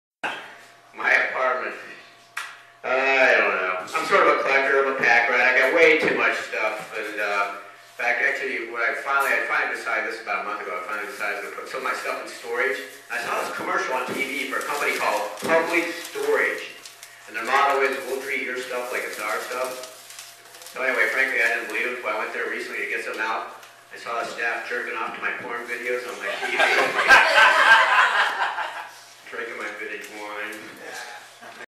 Public Storage VOL
I'm a collector. I've got way too much stuff. In fact I finally decided to put some of my stuff in storage. Yeah, I saw a commercial on TV for this company called 'Public Storage', and their motto is "We'll Treat Your Stuff Like It's Our Stuff". Well frankly I didn't believe 'em ... until I went there to get something out, and I saw the staff jerkin' off to my porn videos on my TV ... and drinking my vintage wines.